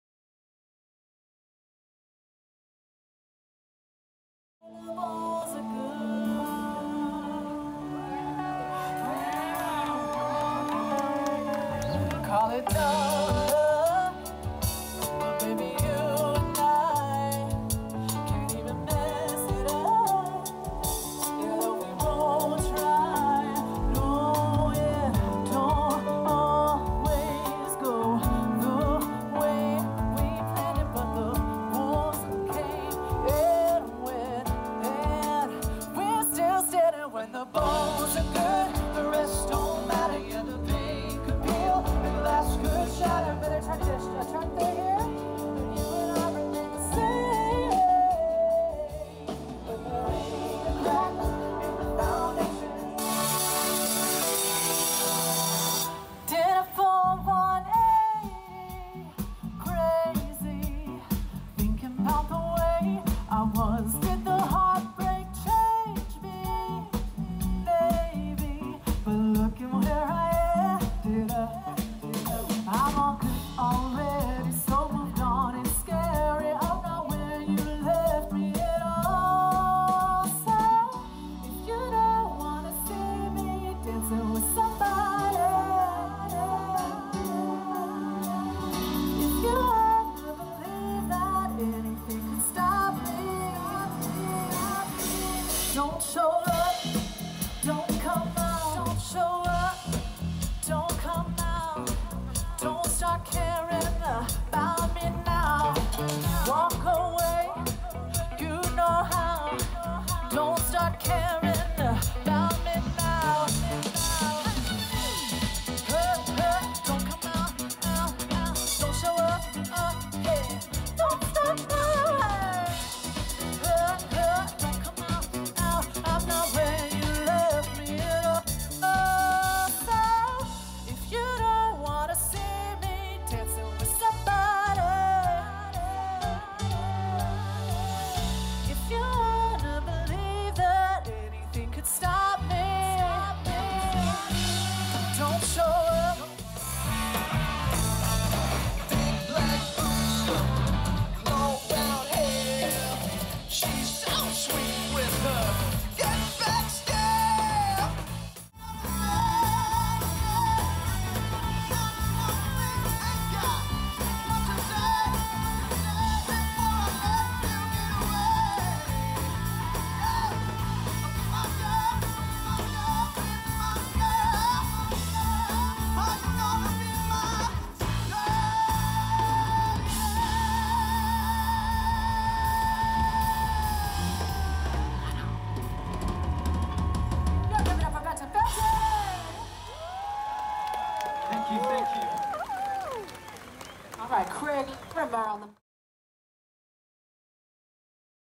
NYC Veterans Day Concert - US Air Force Band Full Spectrum - Washington Square Park - Nov 20, 2021.
Veterans Day Rock Concert - NYC - United States Air Force Heritage of America Band’s Full Spectrum - Active Duty musicians in an electrifying Show - Washington Square Park - New York - Nov 10, 2021.